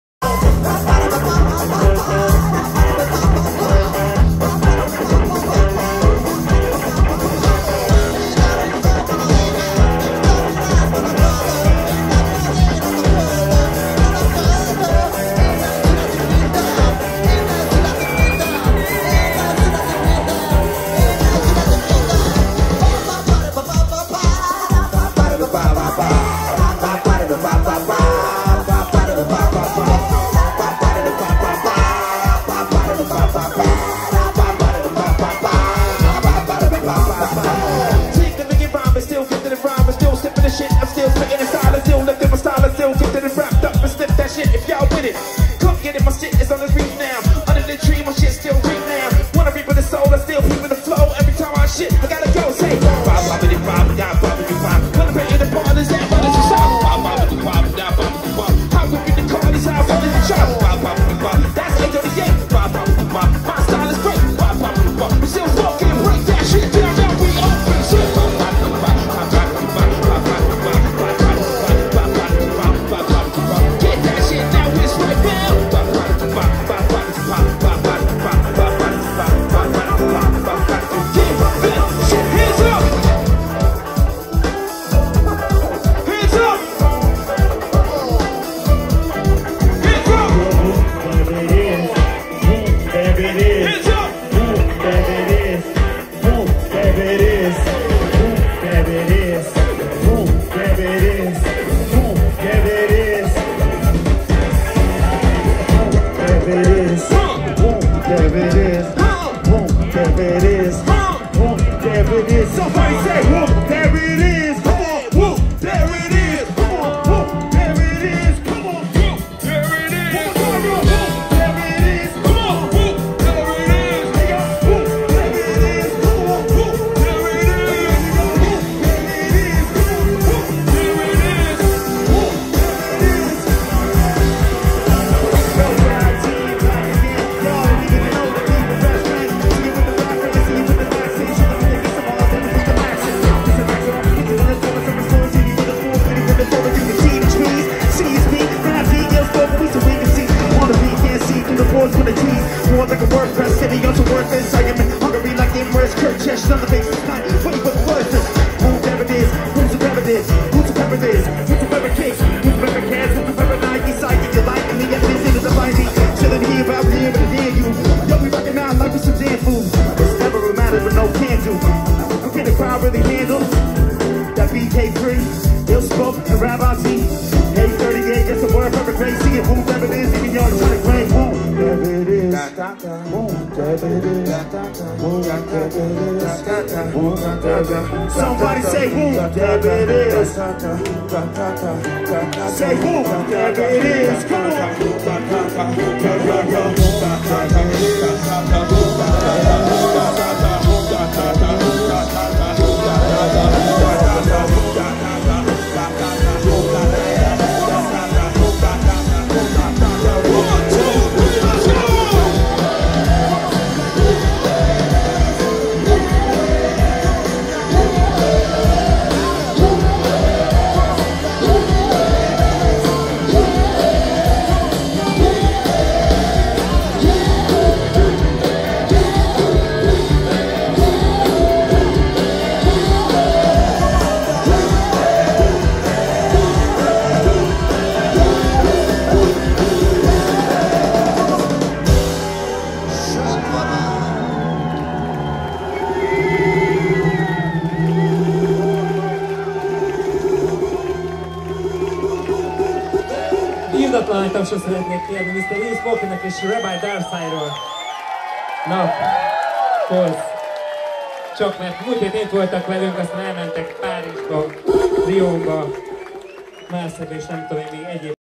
Random Trip feat. Szabó Balázs, Tóth Vera, Méhes Adrián, Szepesi Mátyás @ A38
Szabó Balázs (Szabó Balázs Bandája) - ének, hegedű
Tóth Vera - ének
Méhes Adrián (Muriel) - ének
Szepesi Mátyás (Konyha, Magashegyi Underground) - gitár
Fehérvári Attila (Special Providence, Black-Out) – basszusgitár
Premecz Mátyás (Kéknyúl, Fábián Juli & Zoohacker) – billentyűk
DJ Q-Cee (Vinyl Warriorz) - lemezjátszó/scratch, beatbox
Delov Jávor (Turbo) - dob
iLLspokinn /US/ - rap
Rabbi Darkside /US/ - rap